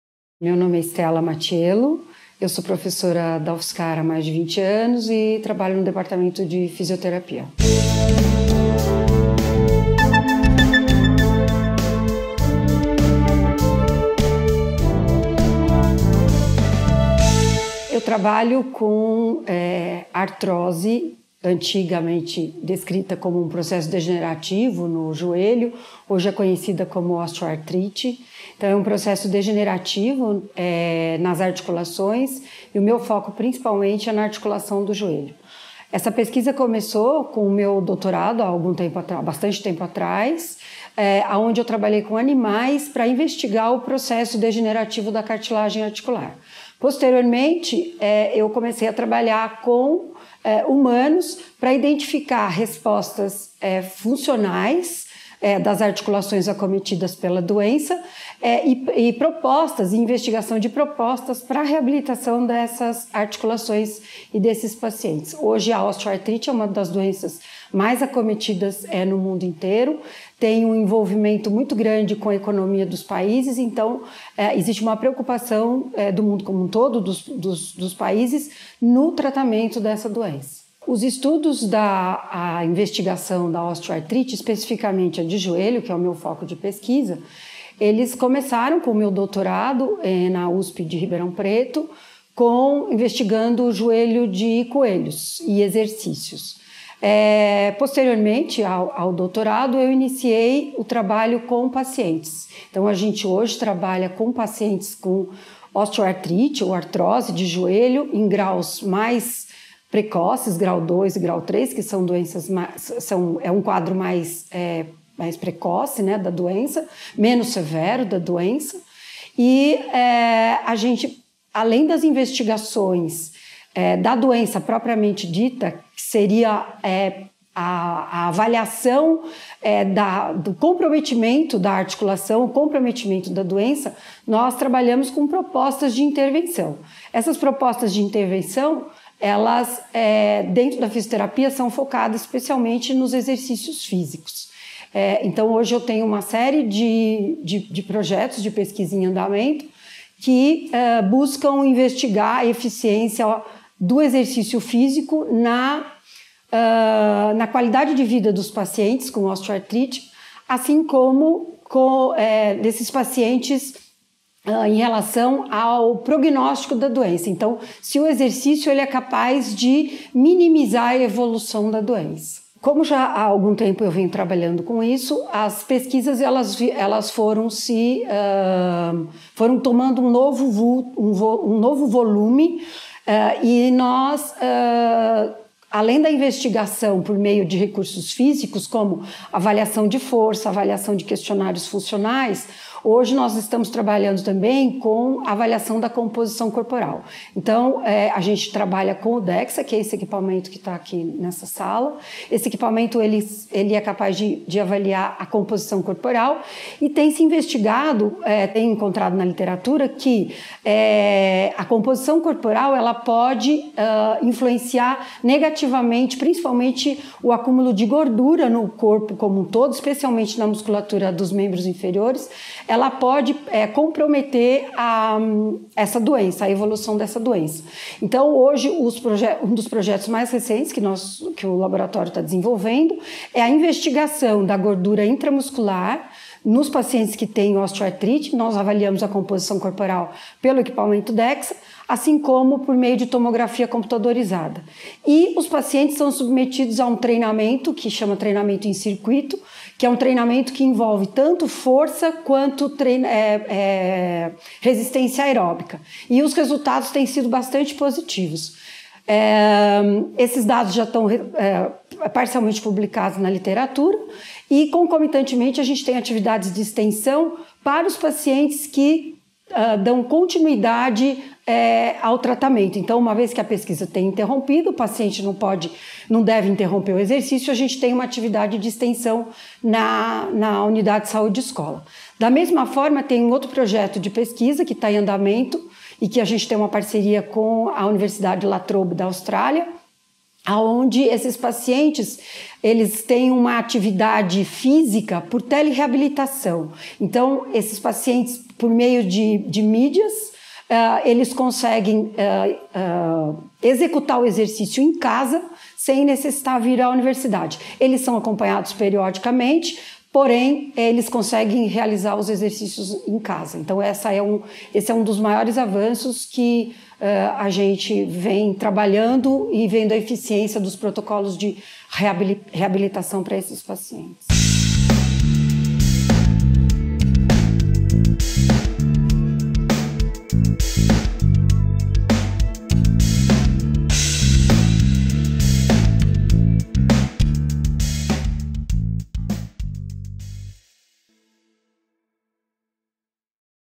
Osteoartrite nos joelhos é tema de pesquisa na UFSCar
Stela Márcia Mattiello, docente do Departamento de Fisioterapia da Universidade Federal de São Carlos (DFisio - UFSCar), fala de sua pesquisa sobre osteoartrite nos joelhos.